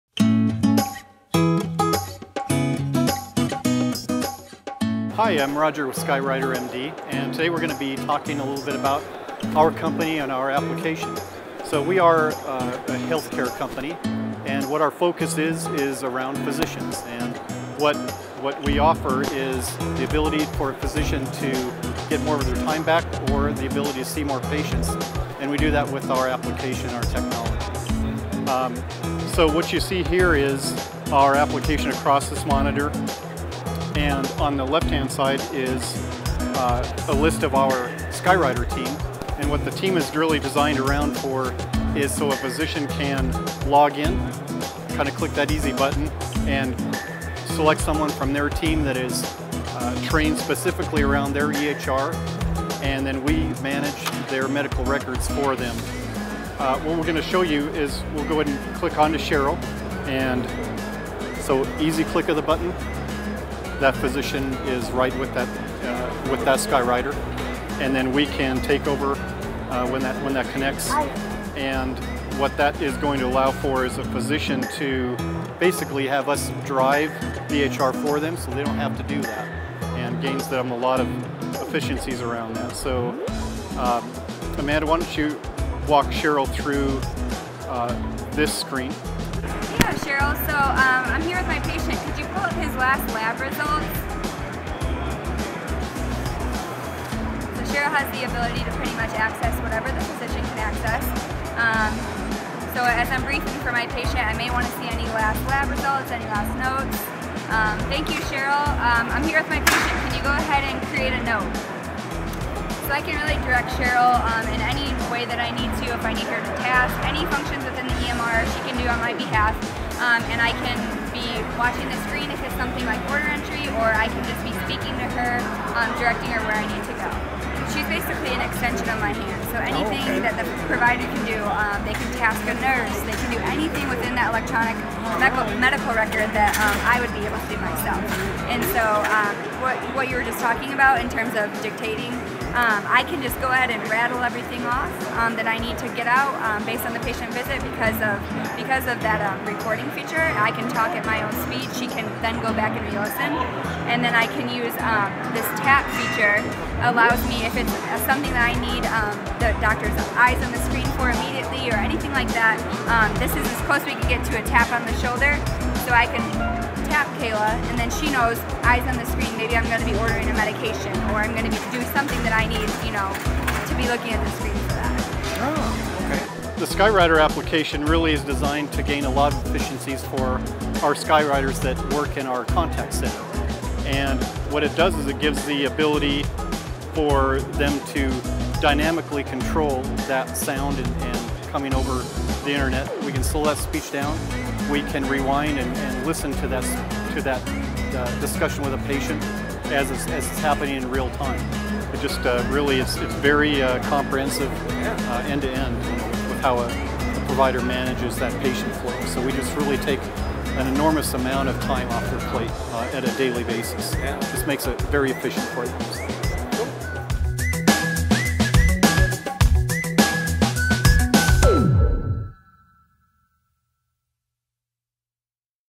Skywriter MD Demo at CenturyLink Tech Forum (Kandy use-case)
Showcasing the Kandy-enabled application from Skywriter MD at the CenturyLink Tech Forum. Denver, CO. September 2-3, 2015. Part of the Real Time Communications Tour